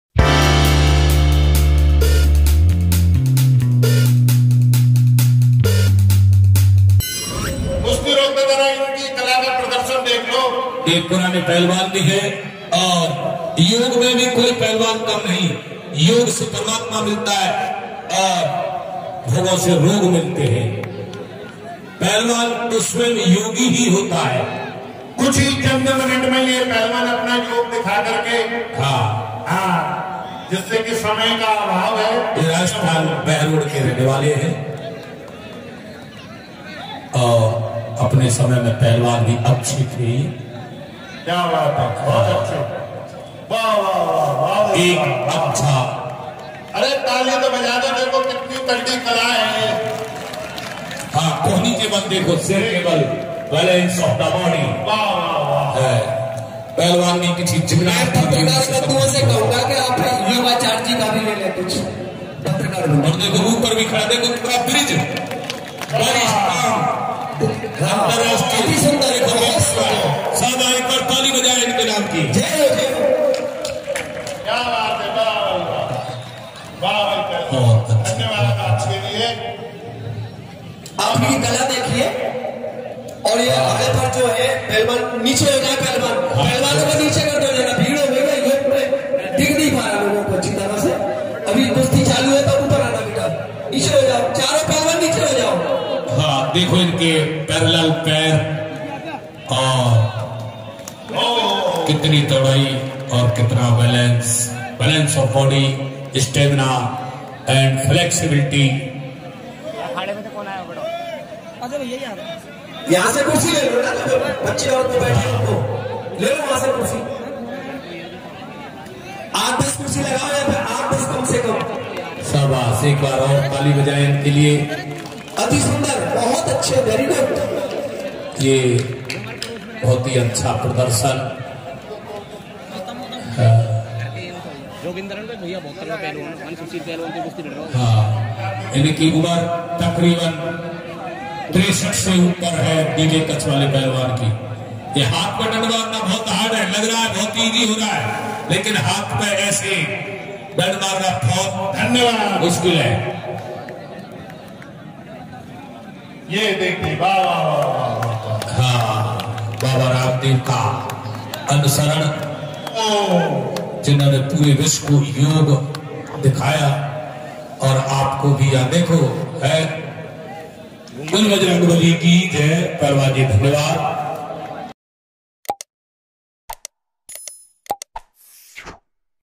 दंगल में पहुंचे योगा गुरु | देशी पहलवान | पहलवानों को स्वस्थ्य रहने का तरीका बताया।
विशाल कुश्ती दंगल में योगा गुरु भी अपना करतब दिखाने के लिए पहुंचे एक से एक बढ़कर योगासन करके योगा के विभिन्न  रूप  दिखाए।
साथ ही  योग गुरु जी ने आज के युवा पीढ़ी का मार्गदर्शन किया।
और बताया कि एक पहलवान जितना ज्यादा शारीरिक तौर पर मजबूत रहता है उतना ही लचीला भी।
यही कारण है कि पहलवानों में फुर्ती अविश्वसनीय होती है।